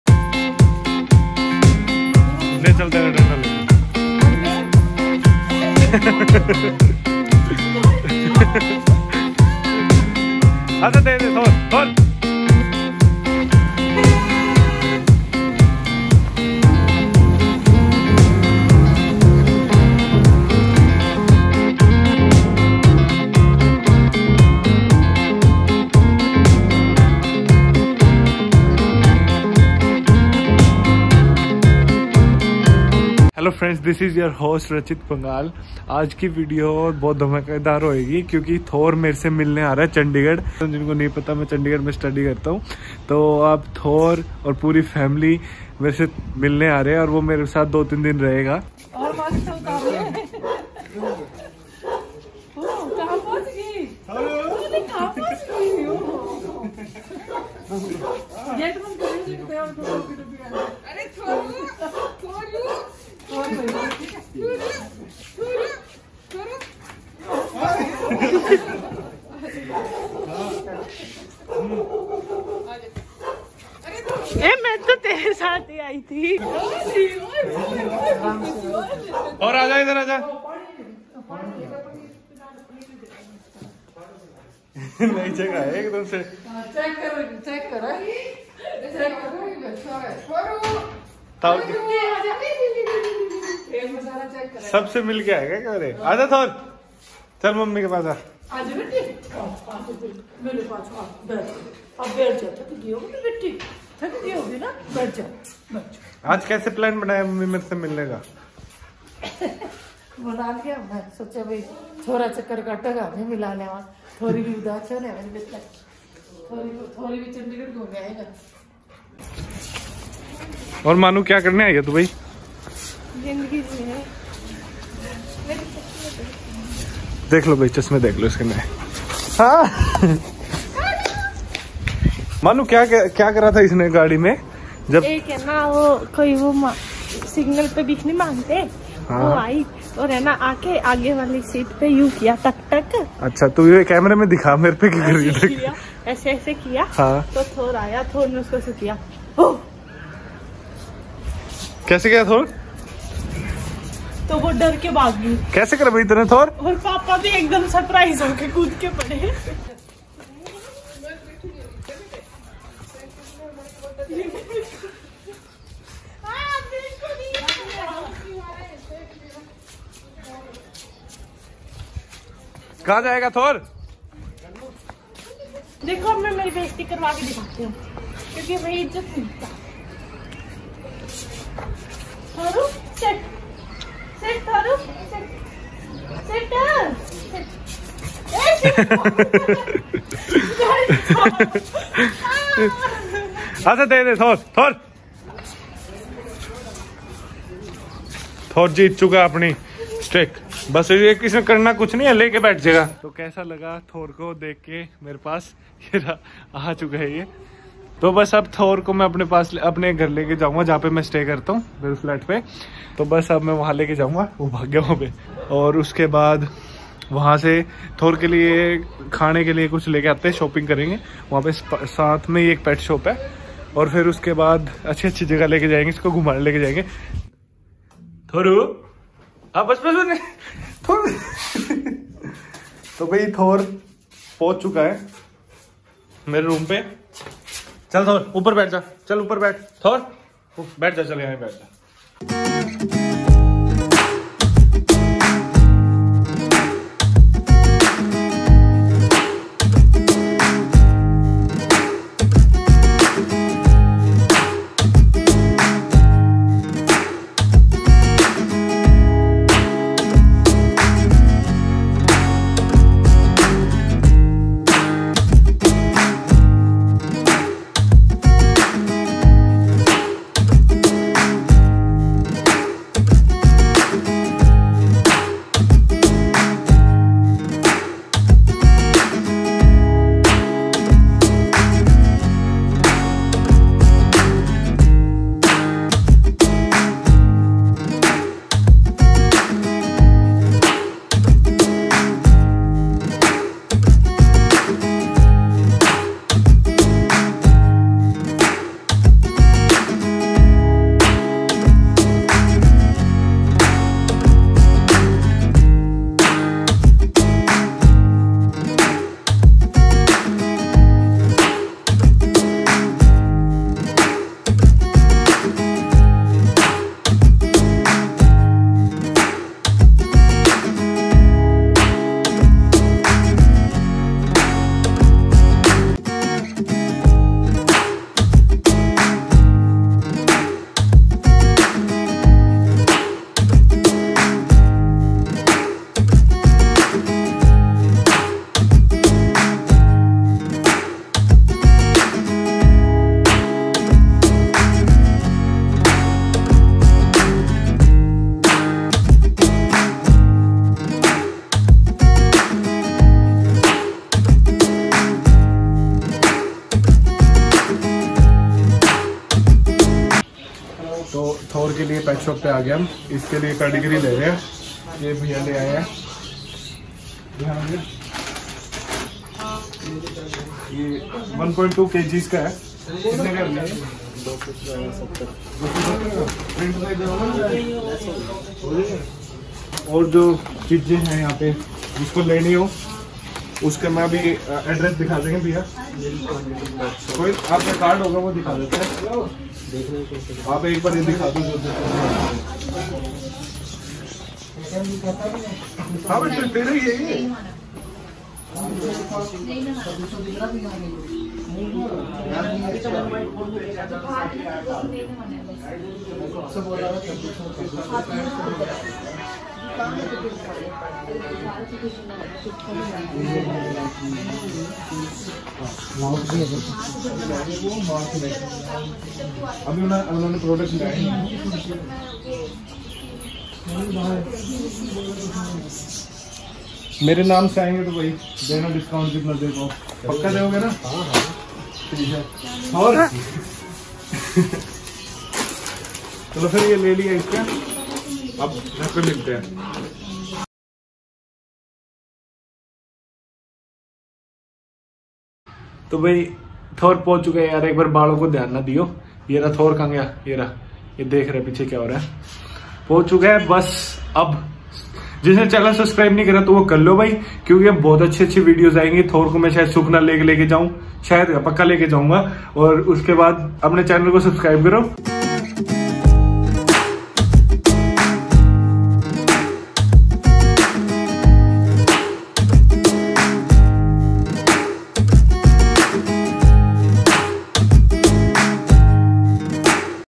Public Reaction: Scared of a Rottweiler? Thor in Chandigarh's Streets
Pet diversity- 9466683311

Camera I use

3 Way Mount Tripod for Camera

Osmo Action Waterproof Case